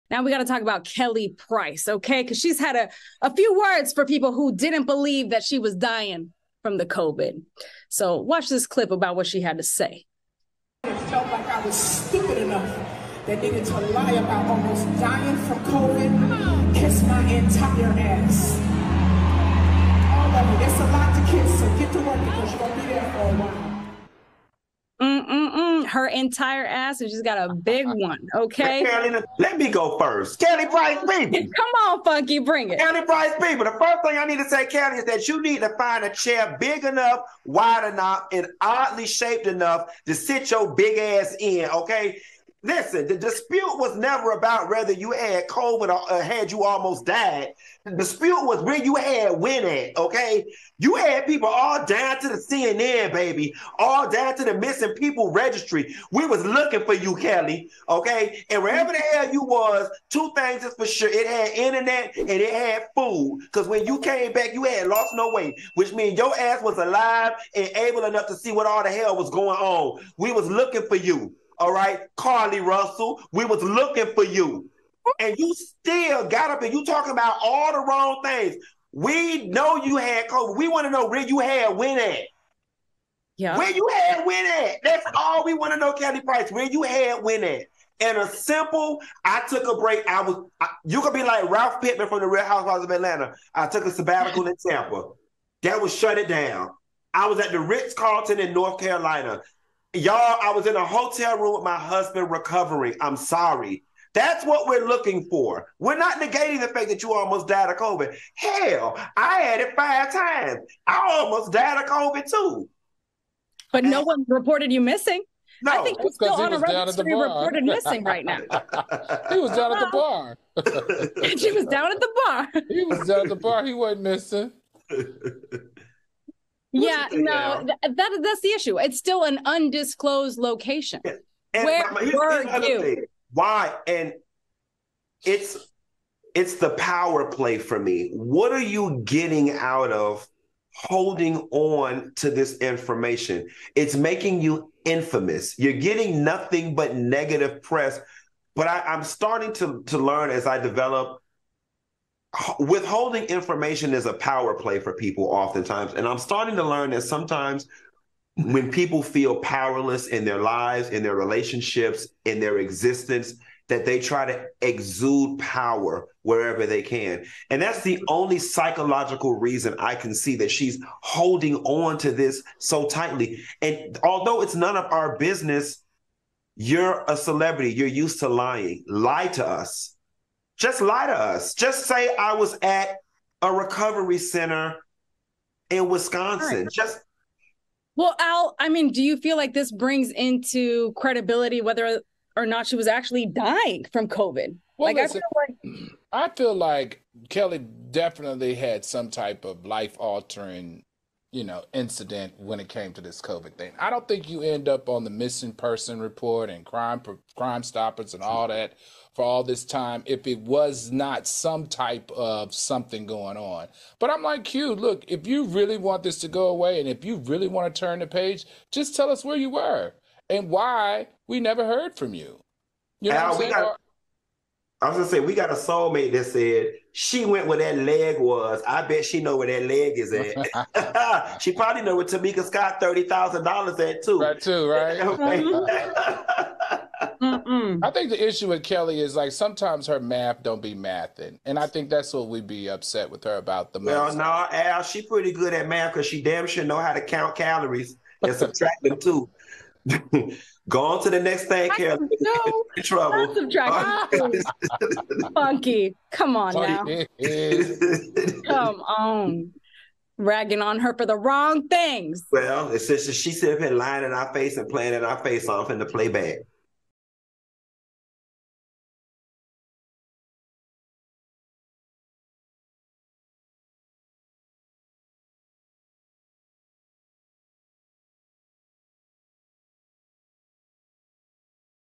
Kelly Price Sets The Record Straight! | Tea-G-I-F
Claudia, Al and Funky are talking about Kelly Price letting us know what she's been up to.